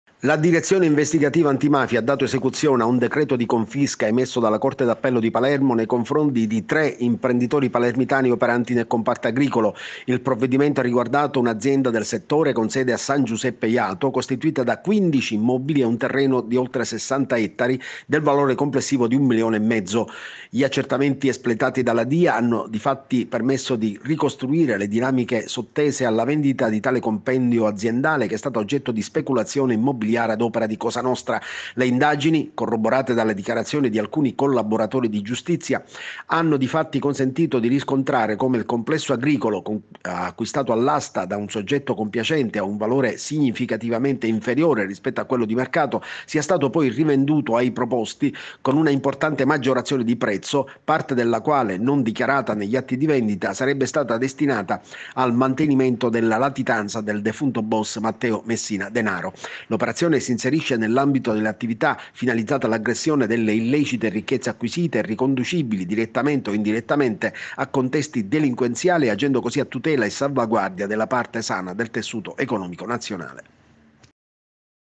LA DIREZIONE INVESTIGATIVA ANTIMAFIA CONFISCA BENI PER CIRCA 1,5MILIONI DI EURO
AD IMPRENDITORE DI SAN GIUSEPPE JATO (PA)